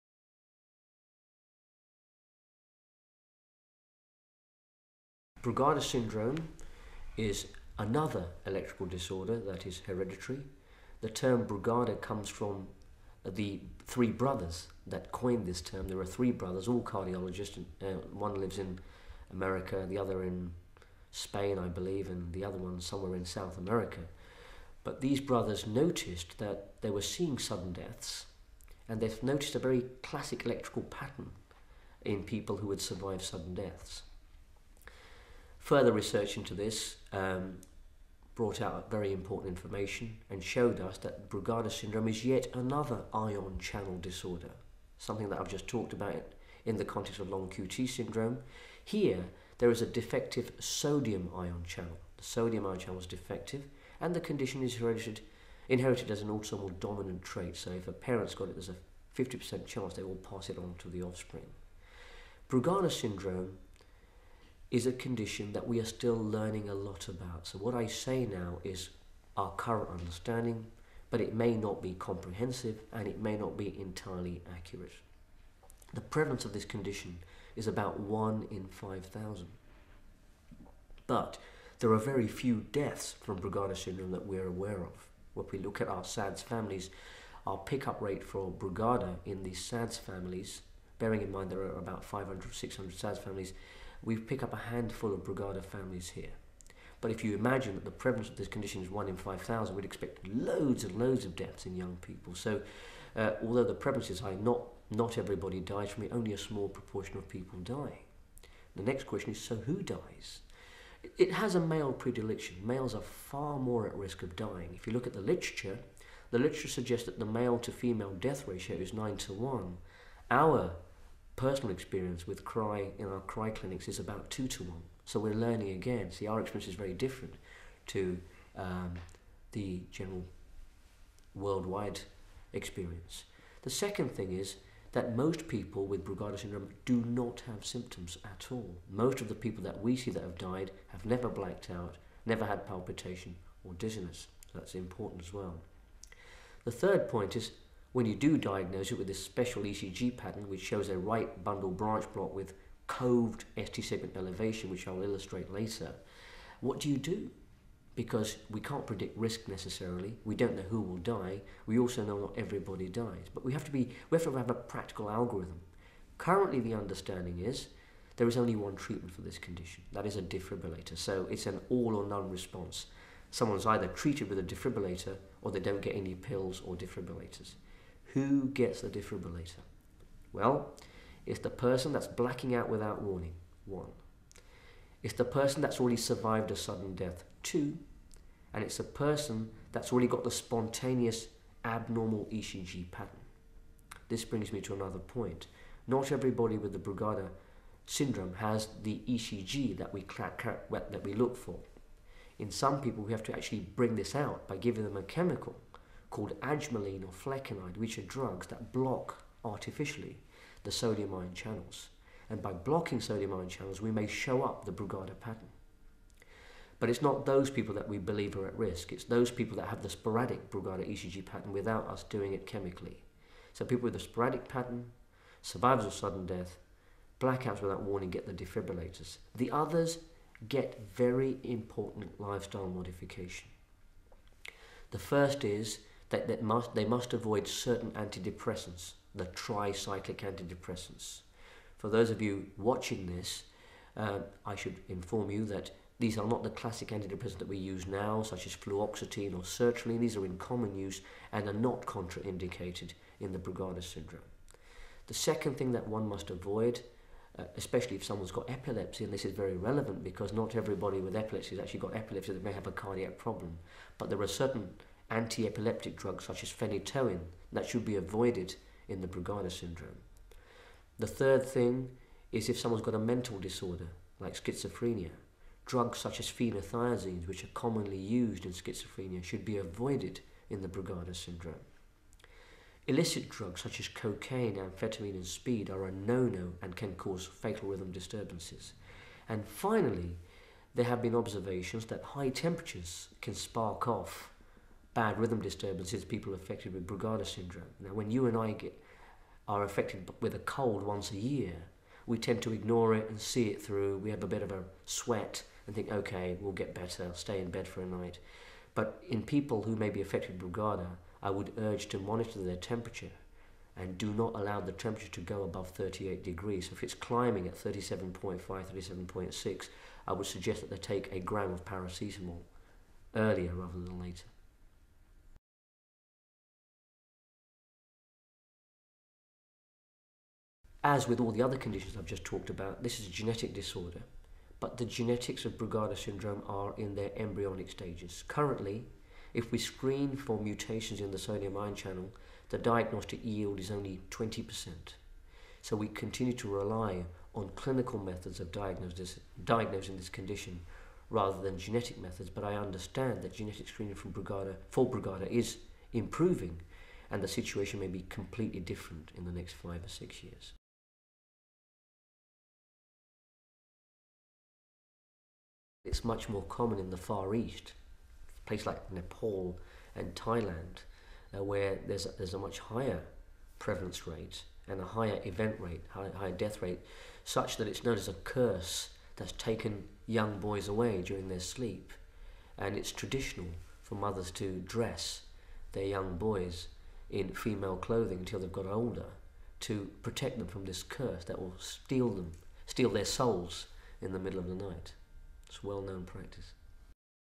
Brugada syndrome
CRY Consultant Cardiologist Professor Sanjay Sharma talks about Brugada syndrome. This video was published in 2011 – please note that the incidence of Brugada is now (July 2017) considered to be 1 in 2,000.